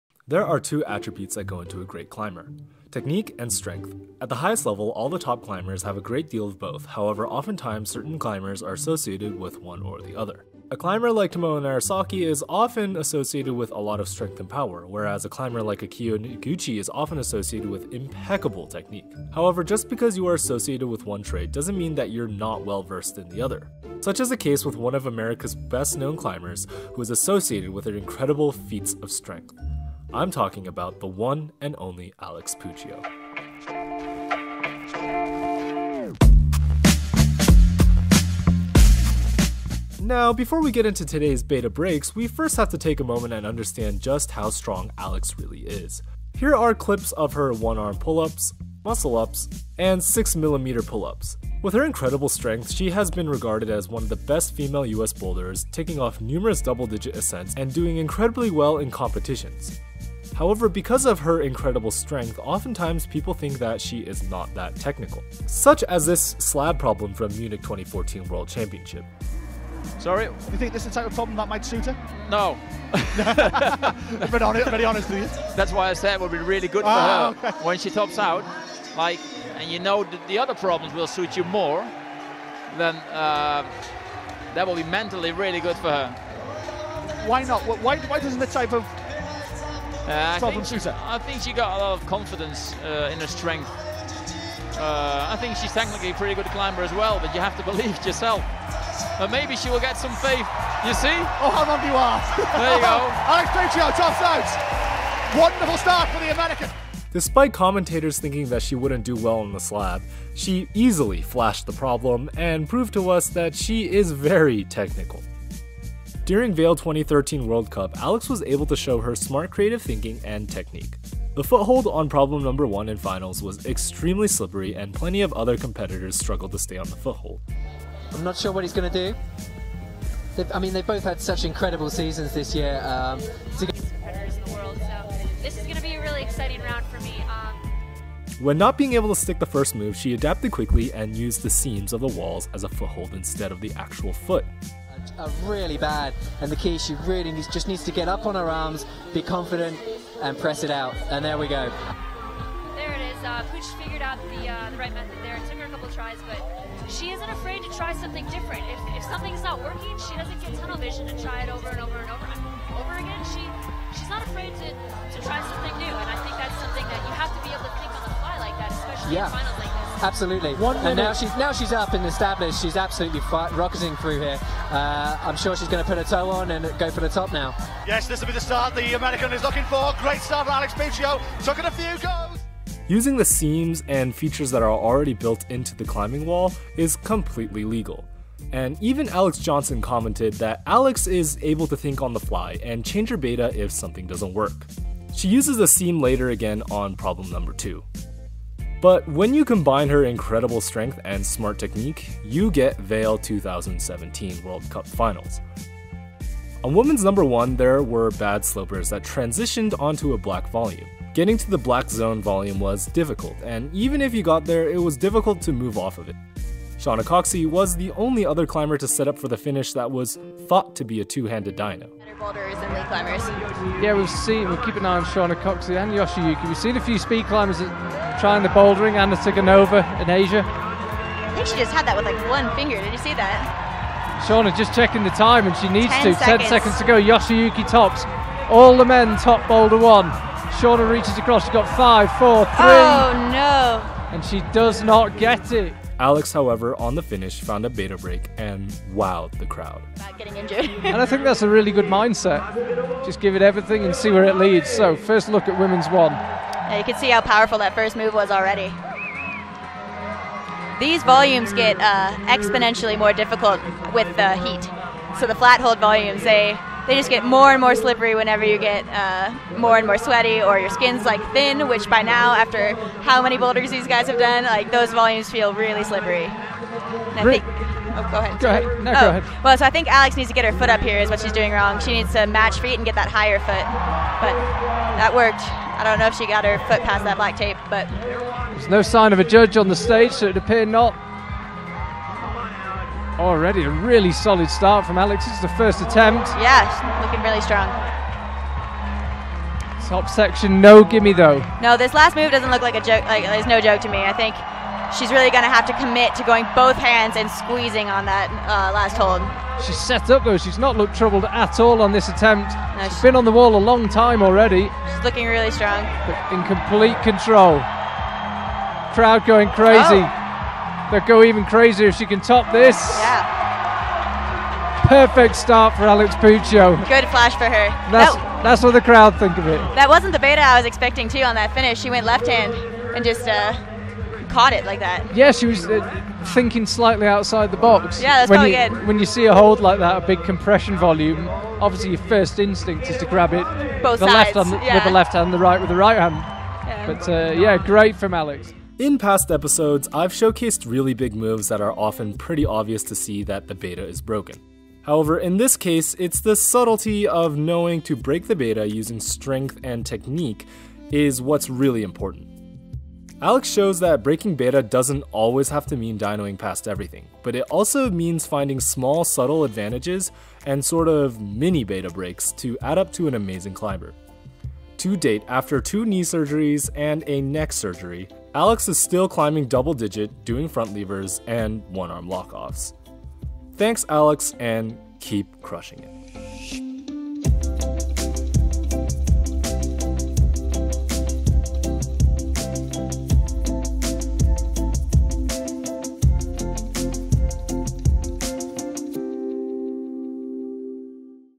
Alex Puccio Finishes Backwards! | Beta Break Ep.7
Alex Puccio is one of the top US climbers to date.  She has an incredible track record of hard outdoor ascents and an impressive competition resume.  During this episode of Beta Break you we break down and understand that she's not just a "strong" climber.  She has allot to show in her technique and adaptive thinking which has made her a successful climber for years.

If you're interested in online coaching DM me!